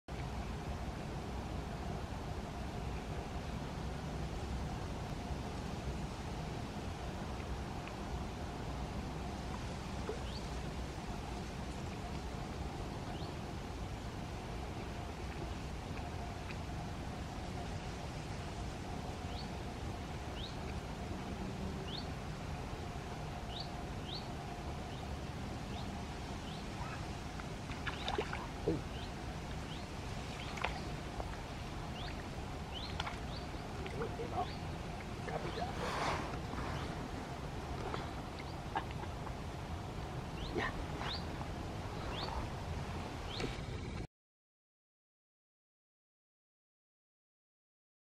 Tanago Fischen - eine schöne Rotfeder
Heute habe ich meine 1,05m Eigenbau-Tanago-Rute mit ein paar besseren Rotfedern in Kontakt gebracht. Die Verbeugung der Rute war deutlich intensiver.